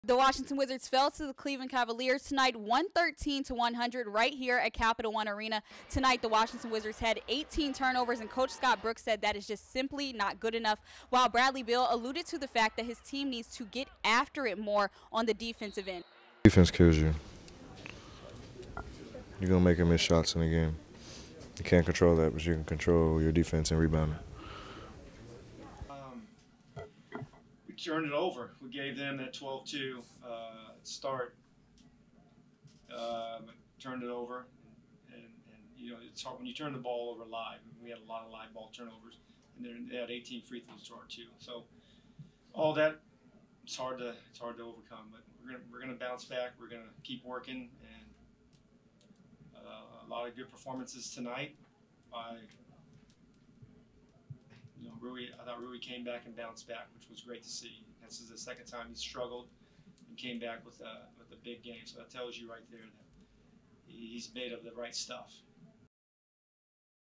DMVSports1: Washington Wizards vs. Cavs Post-Game Recap
The Washington Wizards fell to the Cleveland Cavs 100-113. Washington Wizards,  head coach, Scott Brooks emphasized that the Wizards need to cut back on turnovers. Bradley Beal stressed the importance of playing defense.